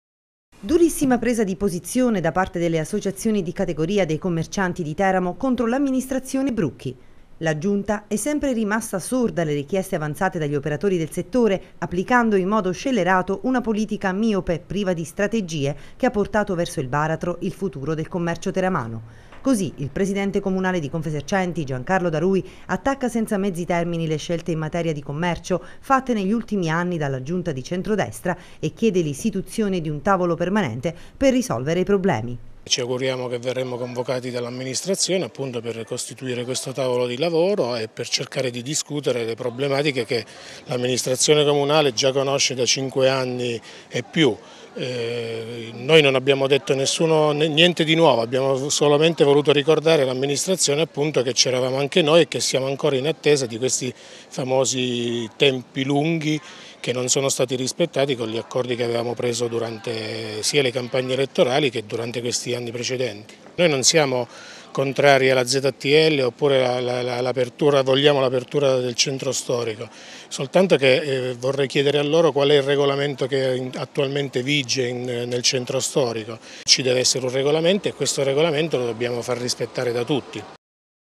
Brucchi e i commercianti
I commercianti teramani si scagliano contro la Giunta Brucchi e accusano l'Amministrazione comunale di essere sorda alle richieste avanzate dagli operatori del settore, portando verso il baratro il futuro del commercio teramano.